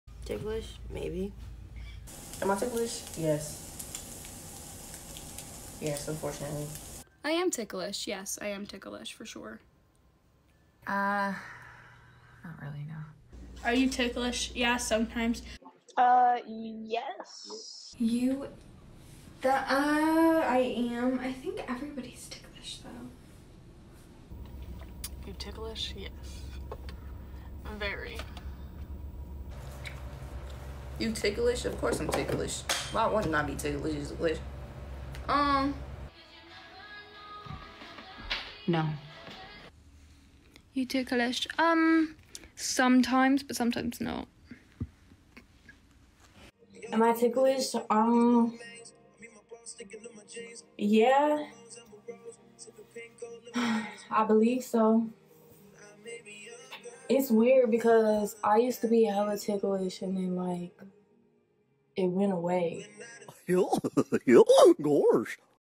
Are You Ticklish?
This video is about me trolling people on TikTok Live.
Remember my videos shouldn't be taken serious in any way, i'm just a troll.
And DON'T HATE the people in the video.